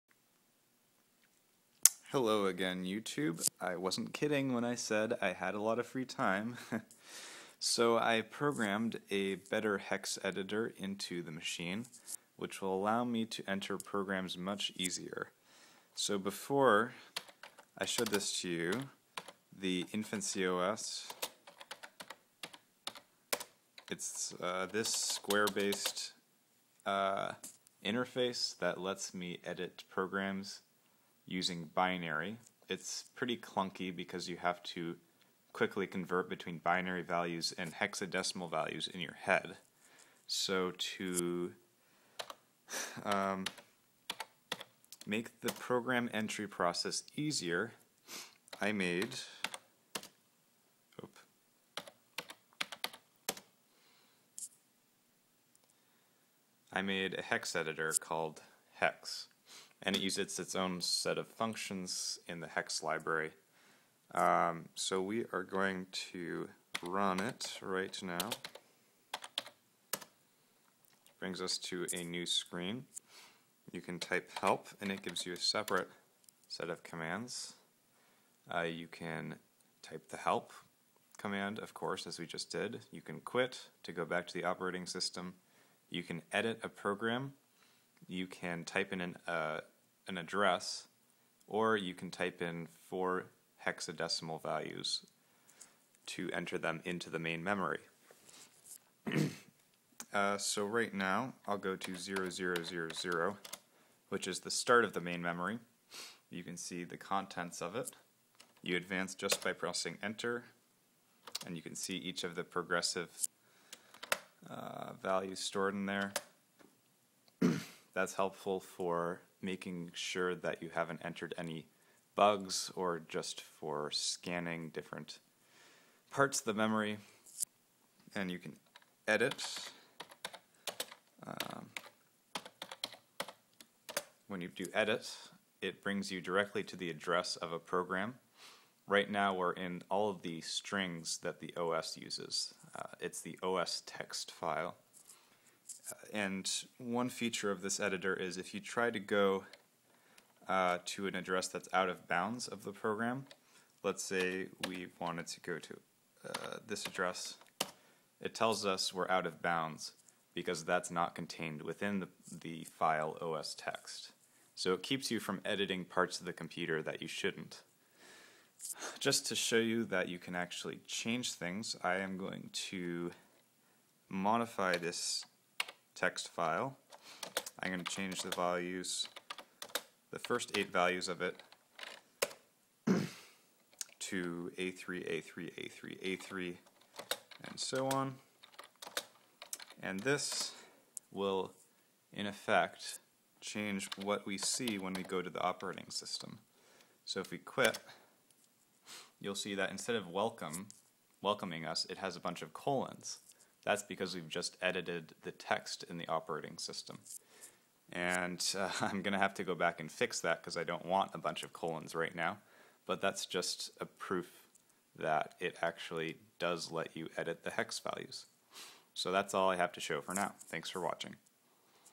DUO Adept TTL CPU + GPU (9)
The DUO Adept is an 8-bit homebrew TTL CPU + GPU I have designed and built. As a challenge I used only basic kinds of logic chips; no microcontroller or video card. It has a black and white TV monitor, keyboard, and speakers, and is generally awesome!